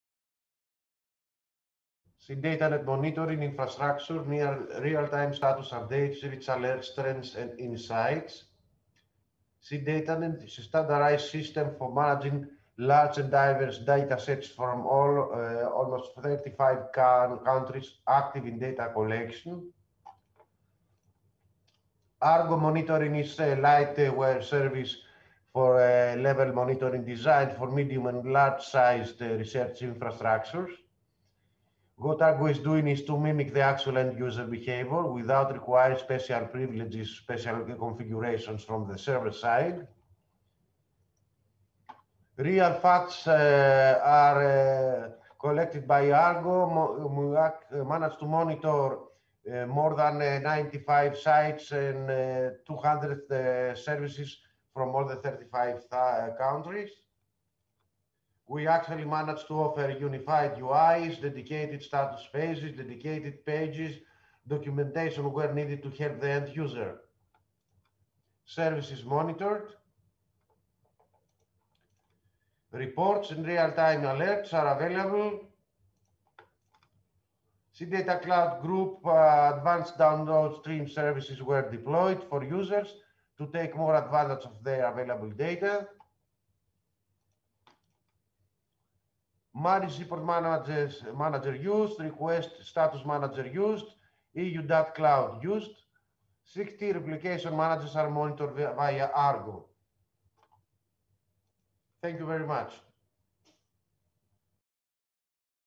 SeaDataNet monitoring infrastructure near real time status updates, rich alerts, trends and insights
Posterpresentation_141

Authors: Angelos Lykiardopoulos (HCMR), Themis Zamani (GRNET), Michail Iordanis (HCMR), Kostas Kagkelidis (GRNET), Kostas Koumantaros (GRNET), Konstantinos Kalkavouras (HCMR) and Athanasia Iona (HCMR)